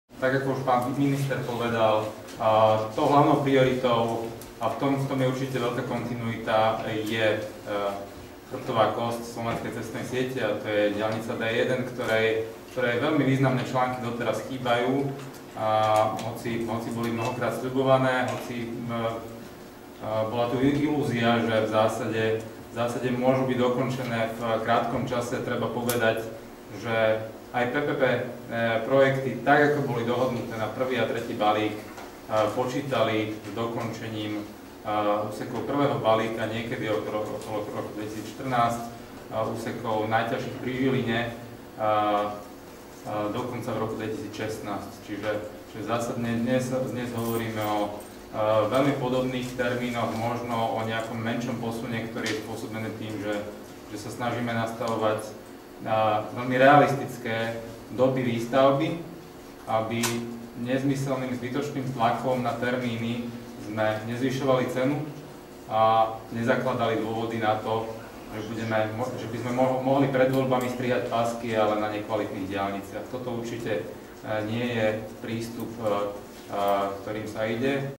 Ján Kovalčík: Program výstavby diaľnic 2011-2014
Riaditeľ Inštitútu stretégii ministerstva dopravy, výstavby a regionálneho rozvoja SR Ján Kovalčík: Nebudeme strihať pásky na nekvalitných diaľniciach. Z prezentácie programu výstavby diaľníc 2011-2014